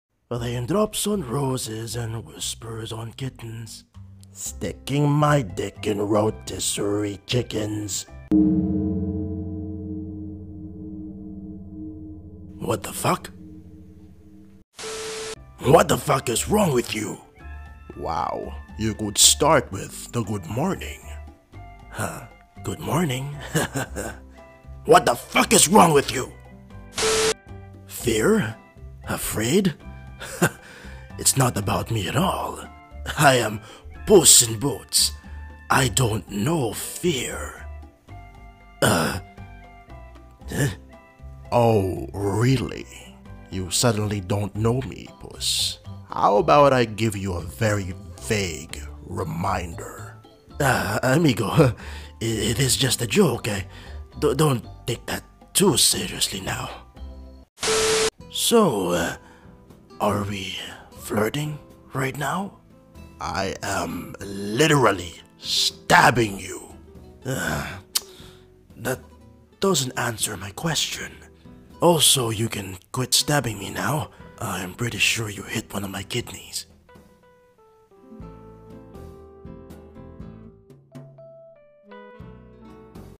Puss in Boots "Flirts" with Death || Puss in Boots the Last Wish (Comic Dub)
A huge warm thank you to the artist Dreacky for giving me permission to dub their comics for today's video!

DREACKY'S TUMBLR!